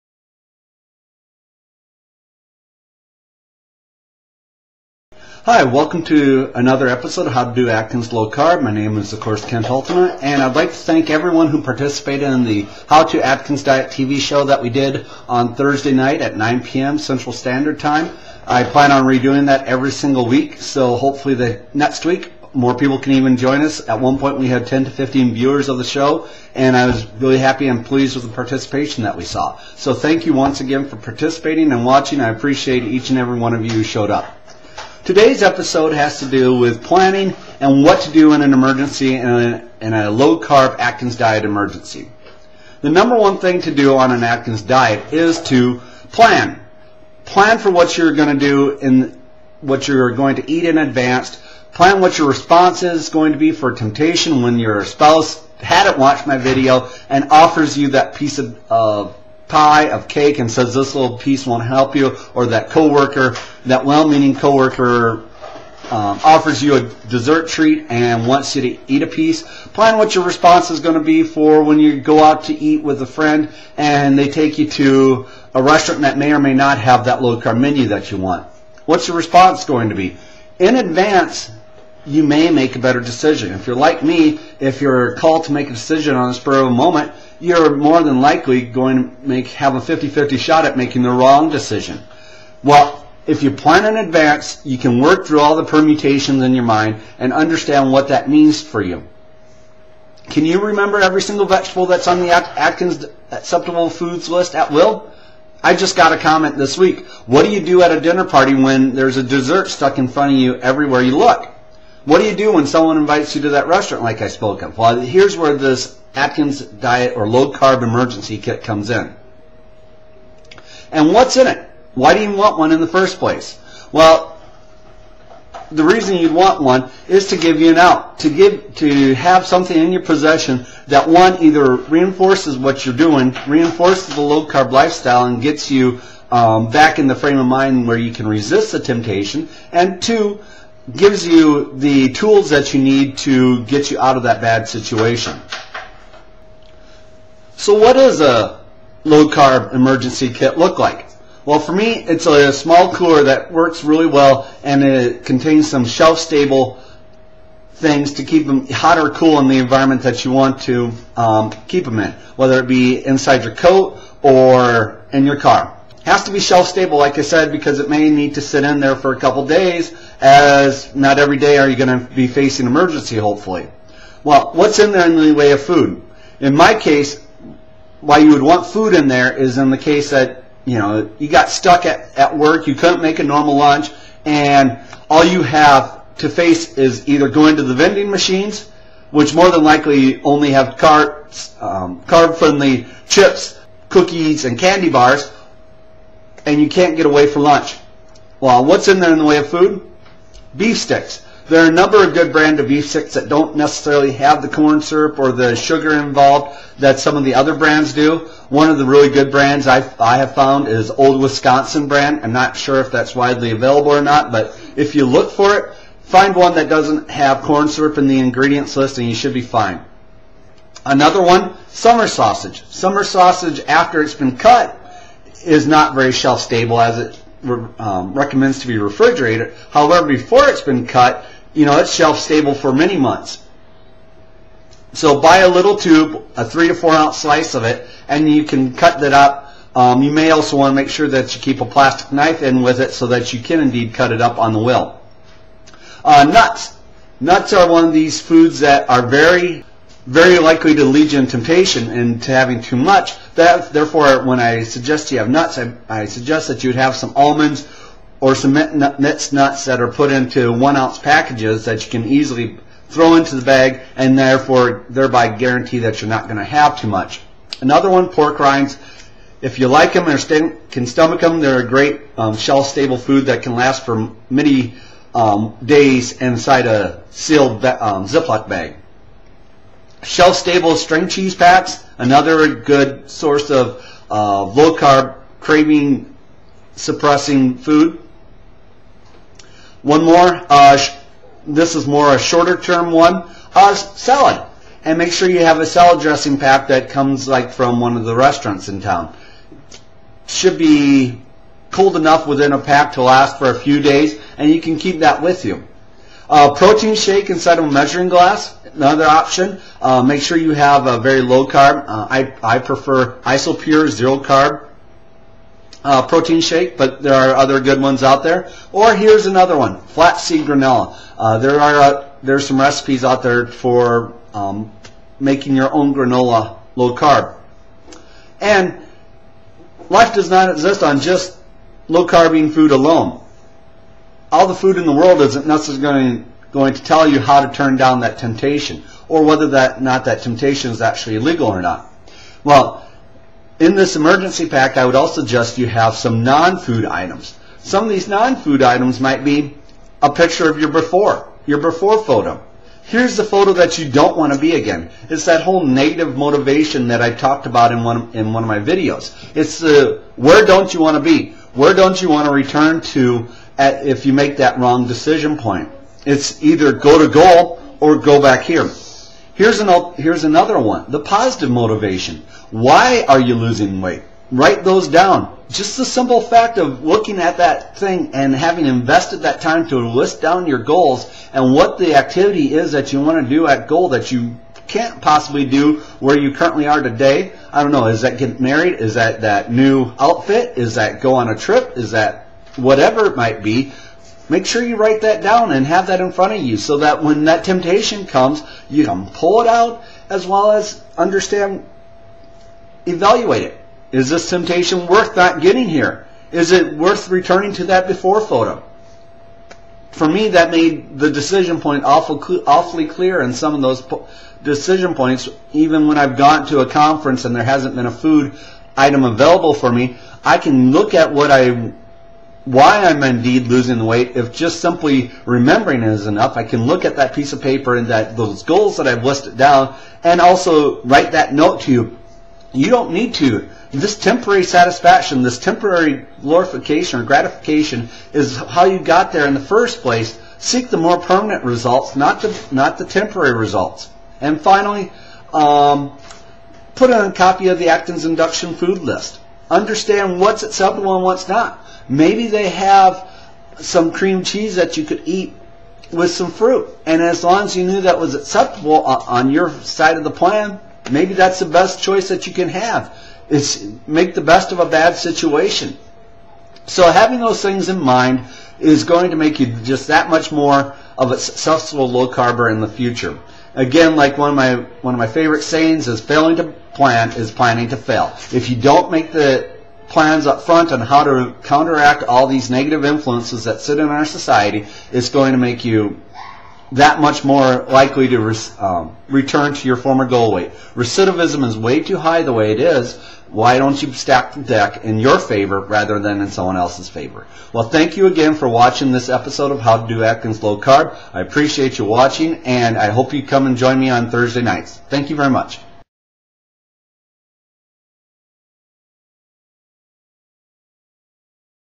Atkins Diet: Low Carb Emergency Kit
In this video, I deal with the issue of planning and how to resist temptation with a tool that should be in every low carbers possession, especially new ones.  That is a low carb emergency kit.  This kit should help resist temptation by reminding you why you want to lose the weight and take care of any hunger pains.

What's in it:
(Food)
* Beef Sticks -- no sugar or corn syrup
* Summer Sausage
* Nuts (portioned 1 oz packages)
* String Cheese
* Protein Shake
* Flax granola / cookie

Non-food:
* Before Photo
* Why you want to lose the weight (Negative / Positive)
* List of the Atkins Induction Acceptable Foods List

Much thanks for ideas of the list to 2big4mysize and Laura Dolson (About.com low carb guide)